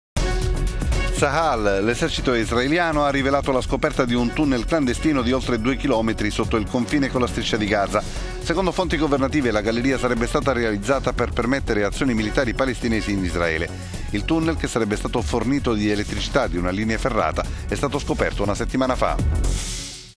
Scoperto da Tsahal un tunnel clandestino tra Israele e Gaza
Tsahal, l'esercito israeliano, ha rivelato la scoperta di un tunnel clandestino di oltre due...

Tsahal, l'esercito israeliano, ha rivelato la scoperta di un tunnel clandestino di oltre due chilometri sotto il confine con la Striscia di Gaza. Secondo fonti governative la galleria sarebbe stata realizzata per permettere azioni militari palestinesi in Israele. Il tunnel, che sarebbe stato fornito di elettricità e di una linea ferrata, è stato scoperto una settimana fa.